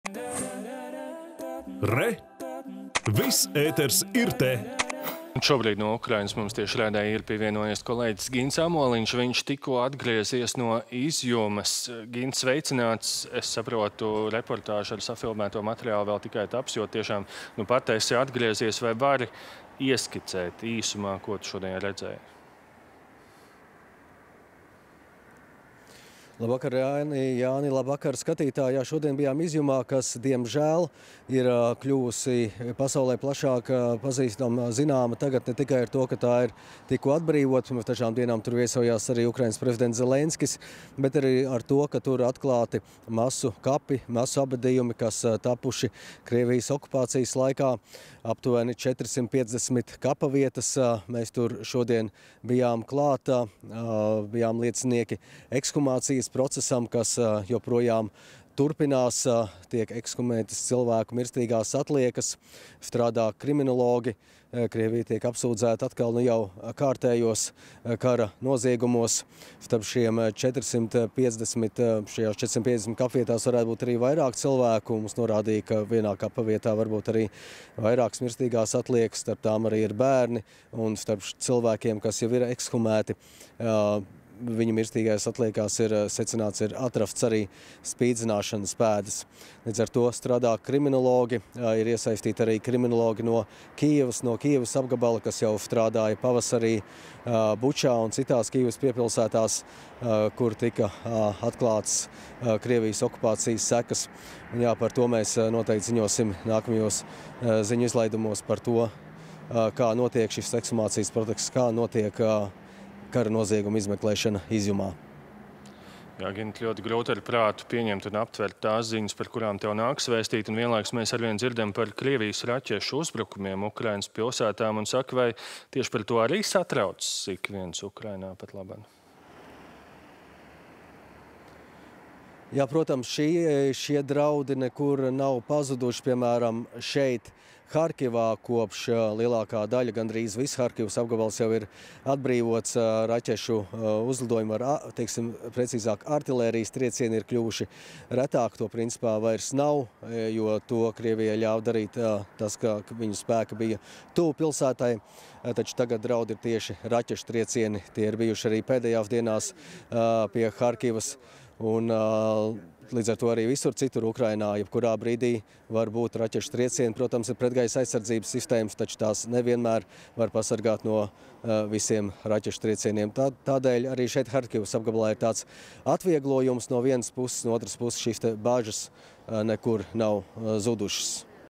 Ukrainā frontē klusuma nav
Un no Ukrainas mums tiešraidē pievienojies kolēģis Gints Amoliņš. Viņš  tikko atgriezies no Izjumas.